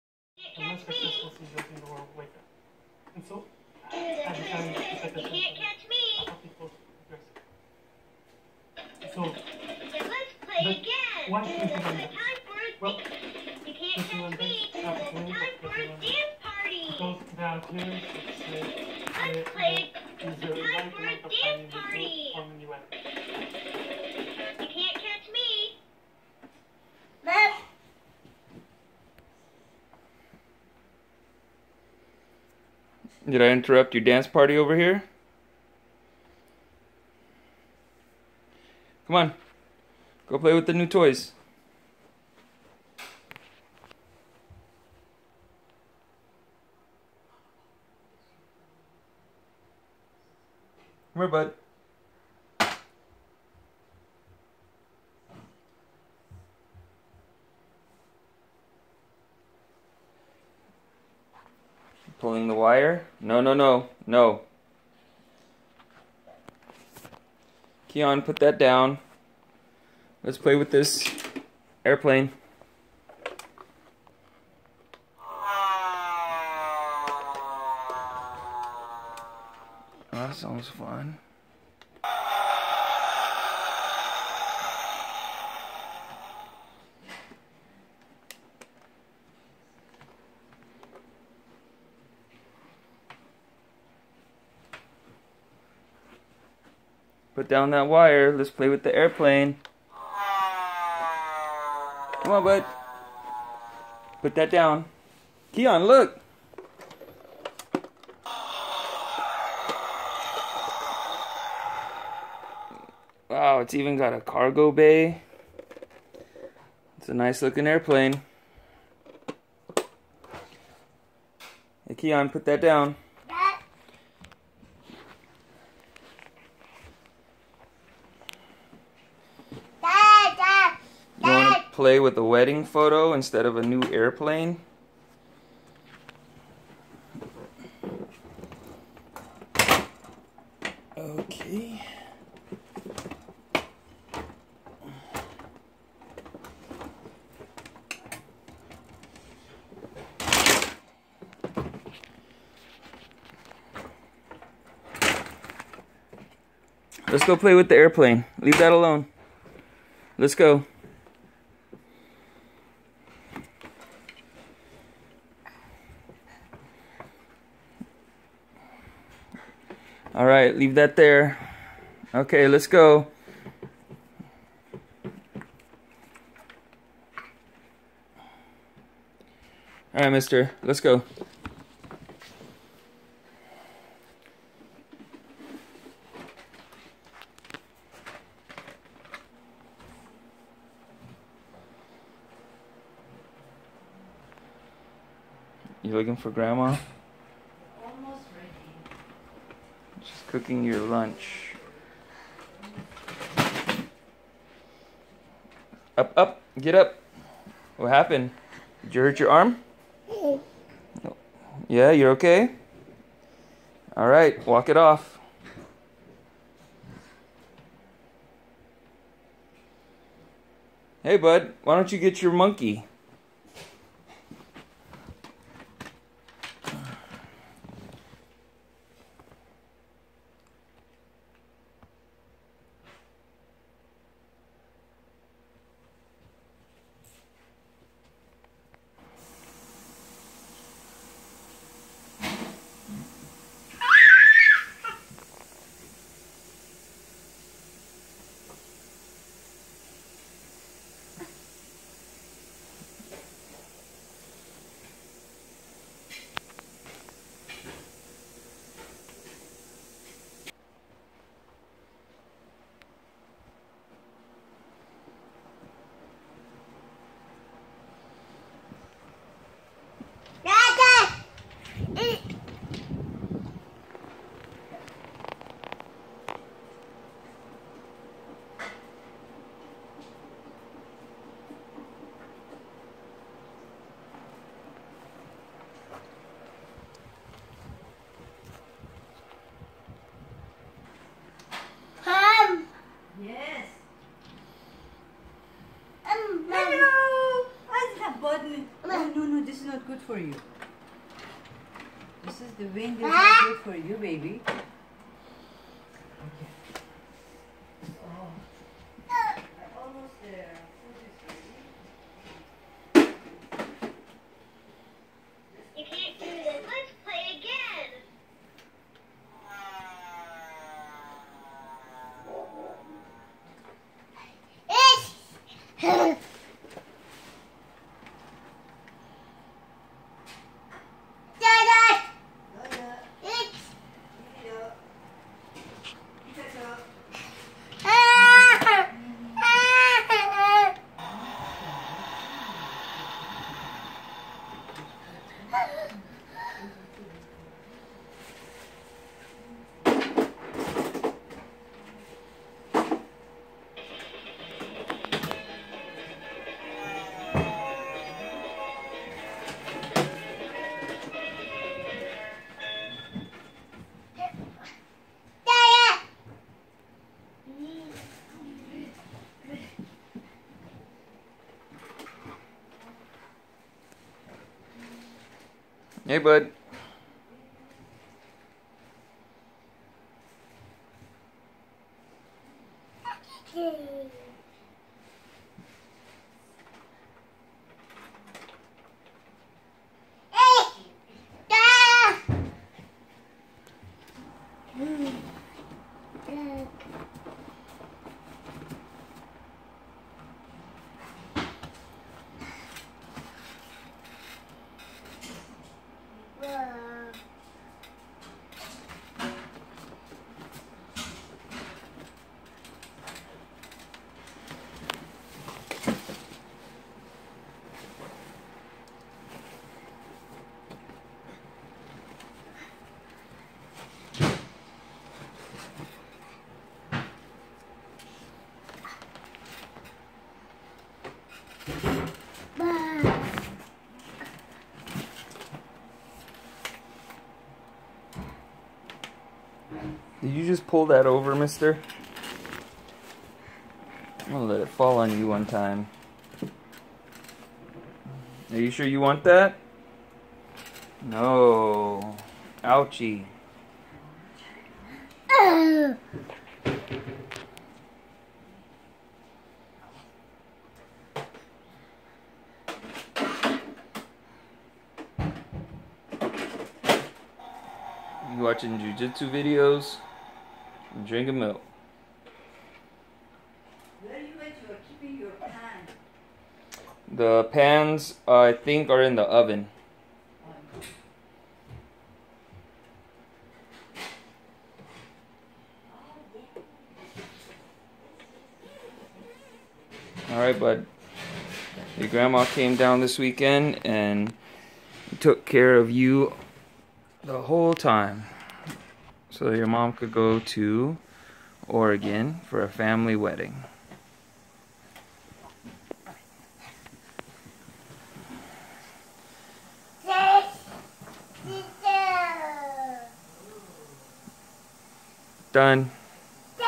16.9mo LivingRoom Playtime with new toys DancingMonkey and Airplane/Chopper
Grandma Sori spending time with Kian while mama is away in Oregon